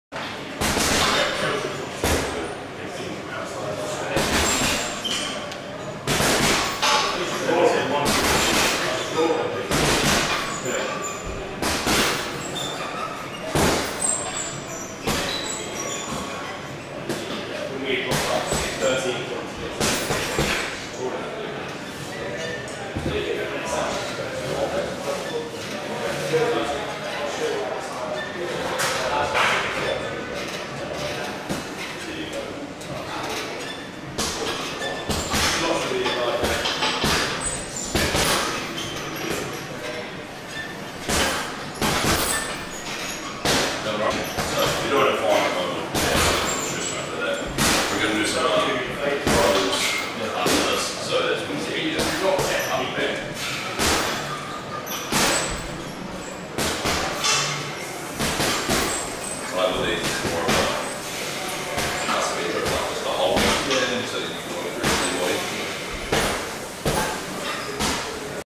3 DB Press Variations
In this video I demonstrate the 3 main variations for the DB Press. The Incline, Flat and Decline DB Press.

If you're trying to grow your chest you should aim to include all these variations in your training. They will enable you to target the different portions of the chest.

Too many people just focus on the Flat DB Press, so make sure you add the other variations to utilize your full potential.

Body Solutions London are a professional team of personal trainers and nutritionists based in Shoreditch, London.